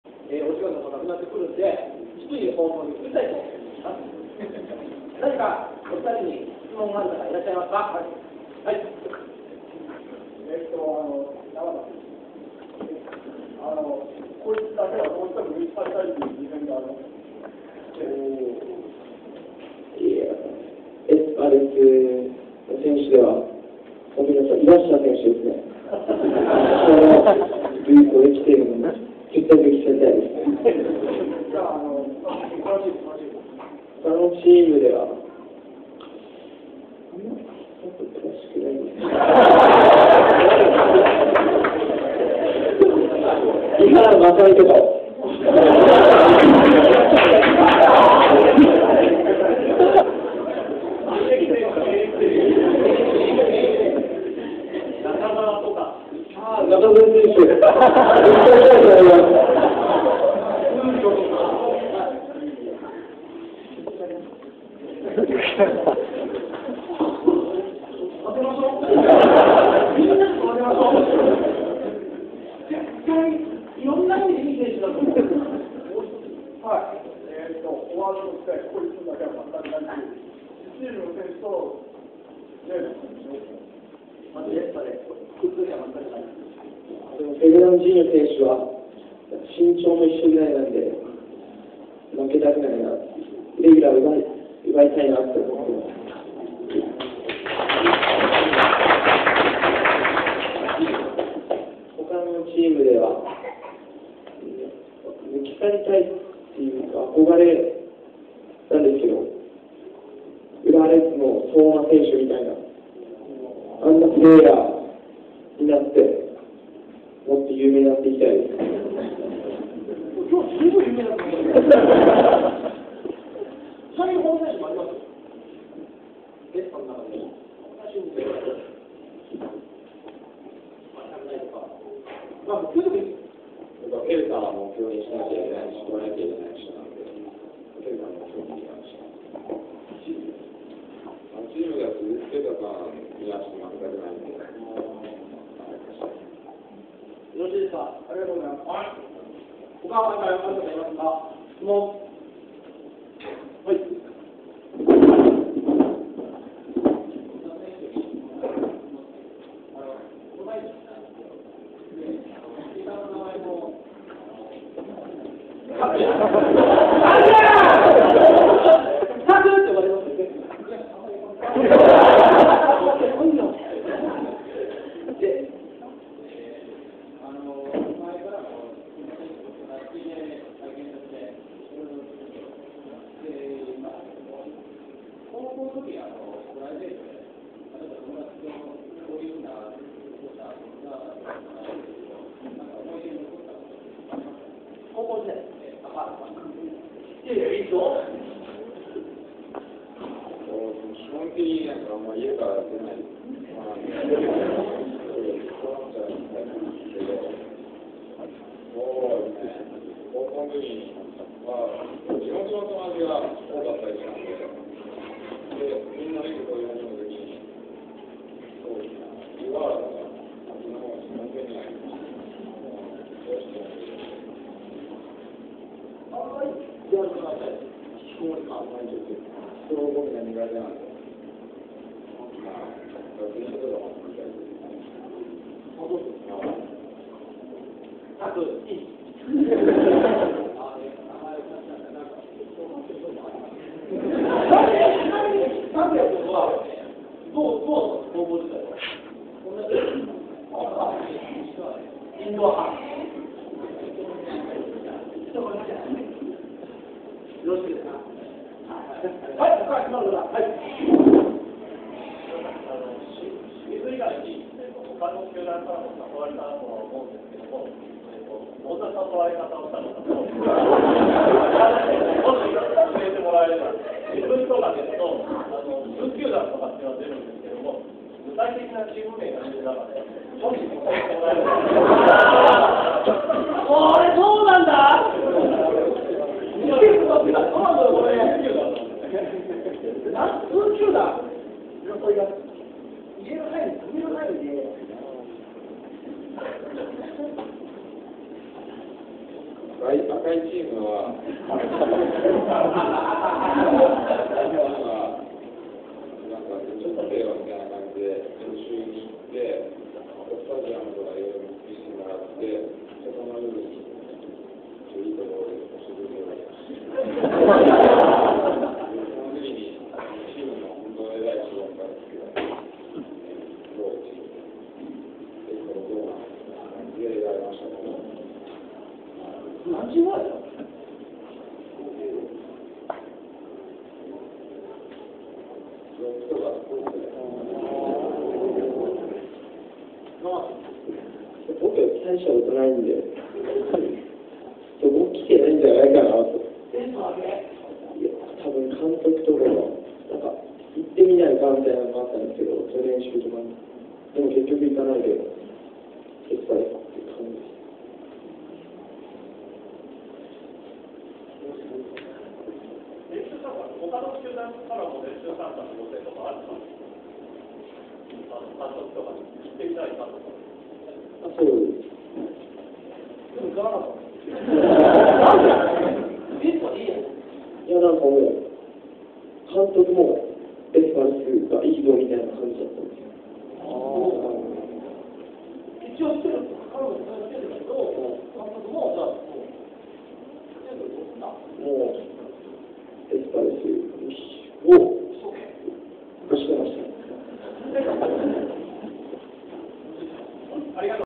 2008 S-Pulse Honda and Nagahata Talk Show (6/7)
Takuya Honda
Yuki Nagahata
and Katsuma Suzuki

at S-Pulse Dream House Shimizu